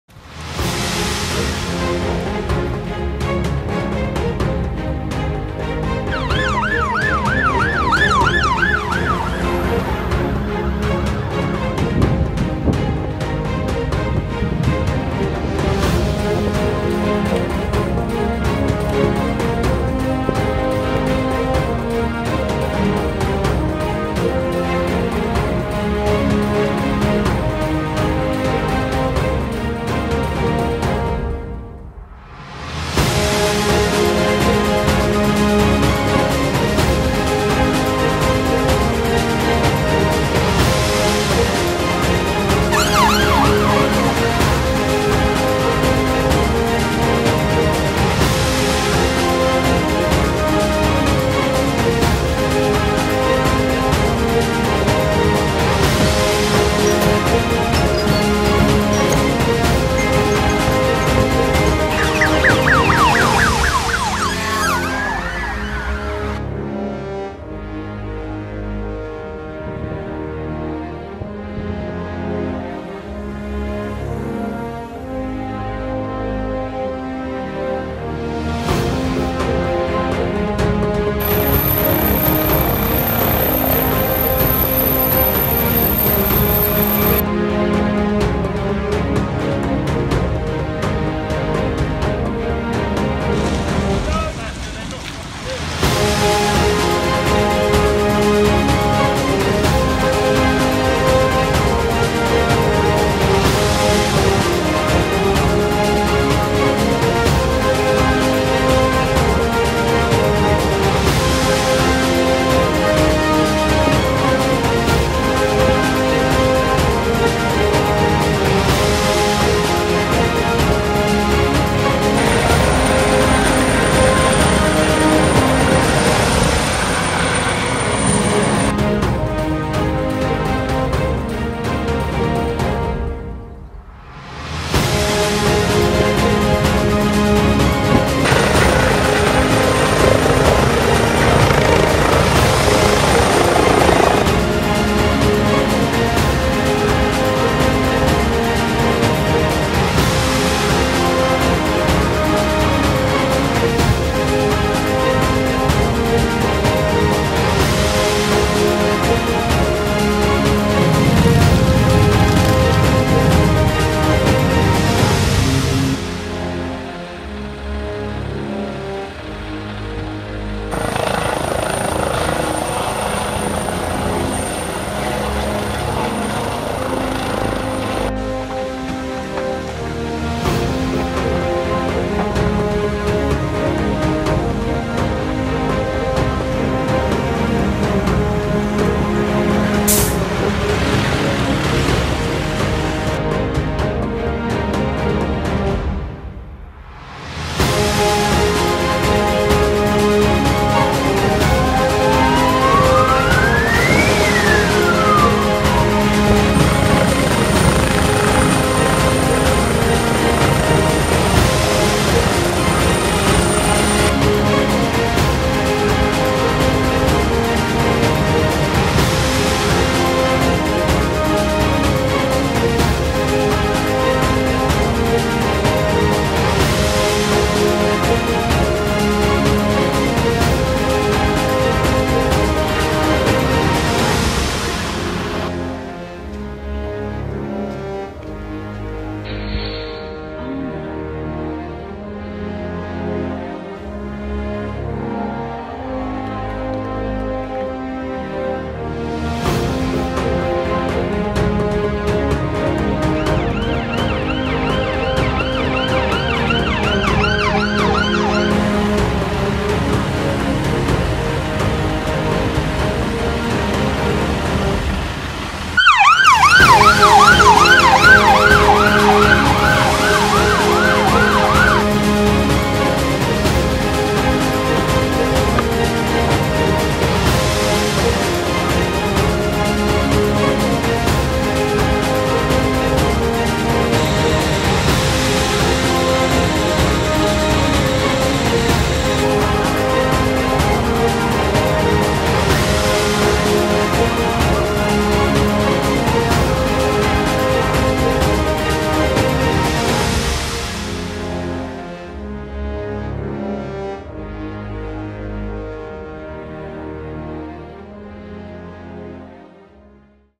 2016 In Motion: Year Review - Island Echo
A look back at some of the Island's breaking news stories from throughout 2016, in motion. These are the stories that made the headlines